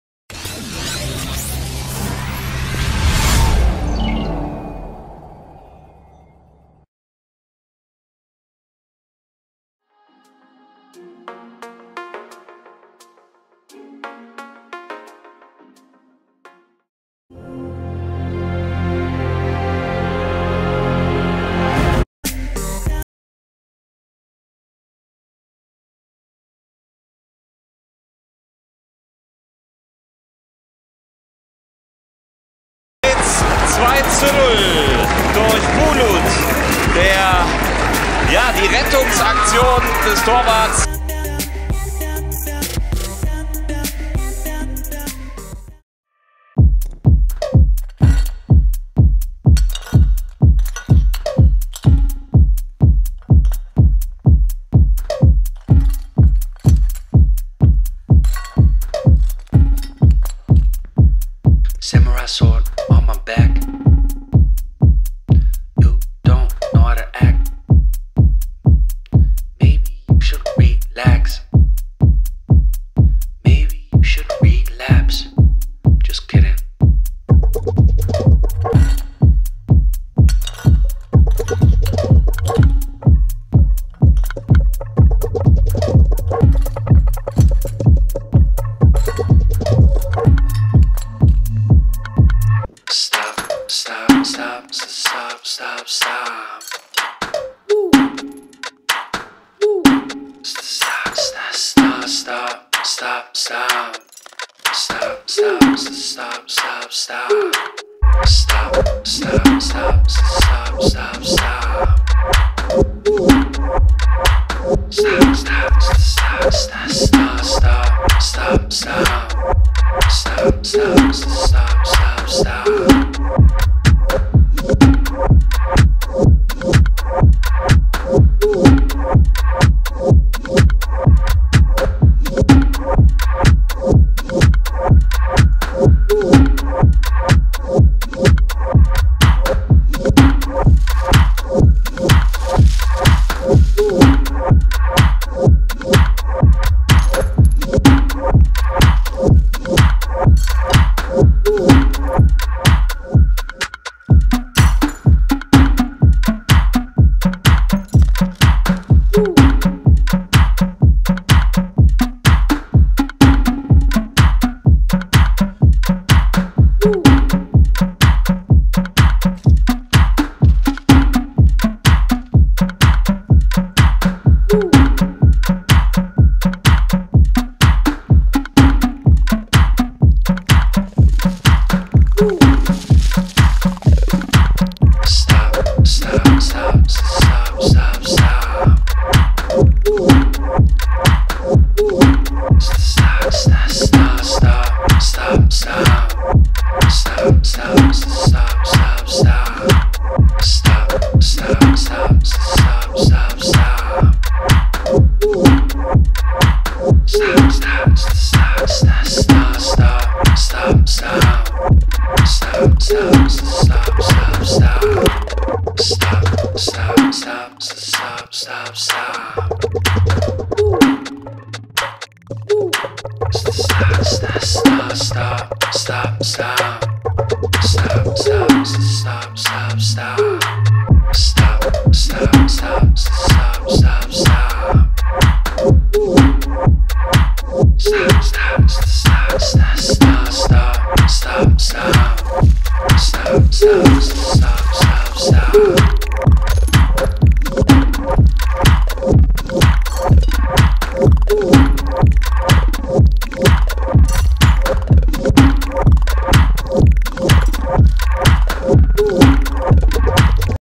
HUSEYIN BULUT ✭ BVB ✭ THE TURKISH SENSATION ✭ Skills & Goals ✭
Hüseyin Bulut is a Turkish professional footballer who plays as a attacking midfielder for BVB U19.